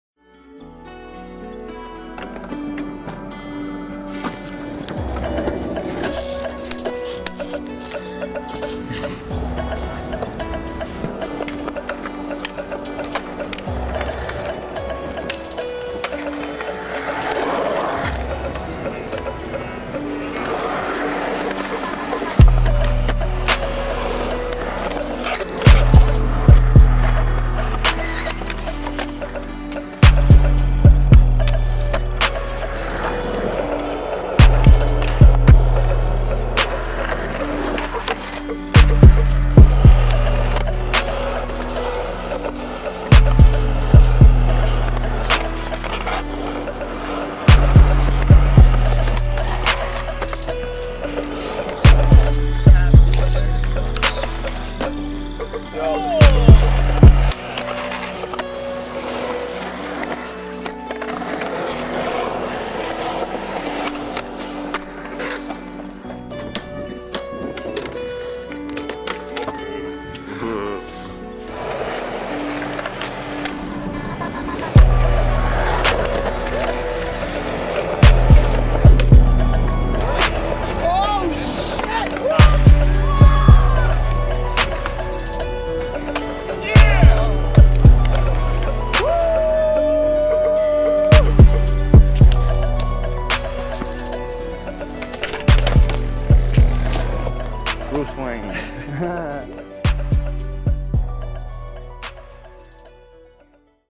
1:45 with Jaden Seitz
computer is finally fixed so I decided to throw some old footy together of the homie, long overdue.. gonna have to film somethin crazy this year!